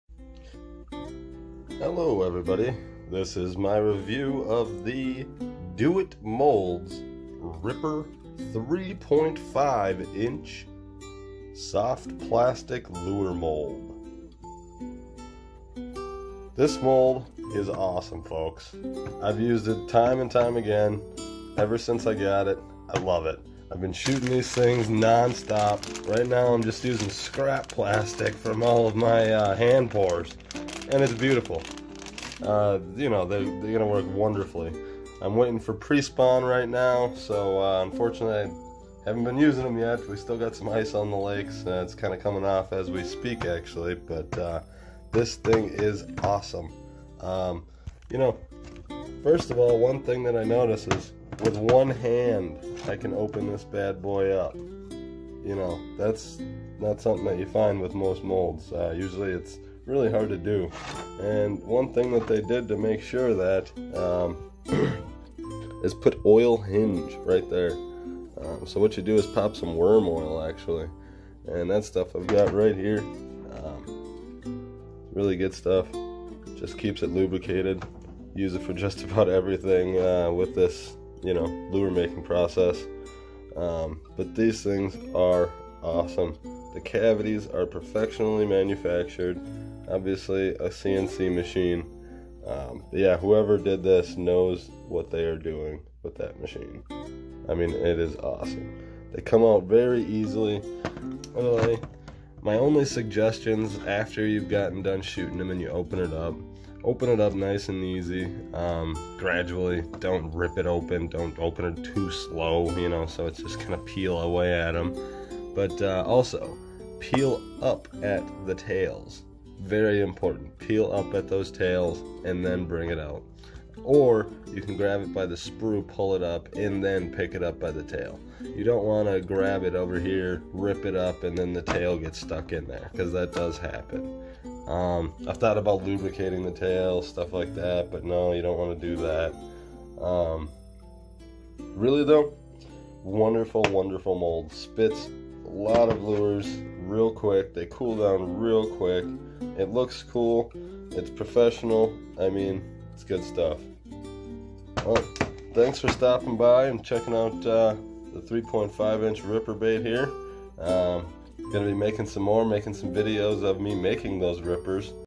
BEST Soft Plastic Lure Mold The RIPPER
Reviewing Do-It Molds 3.5" Ripper Swimbait Mold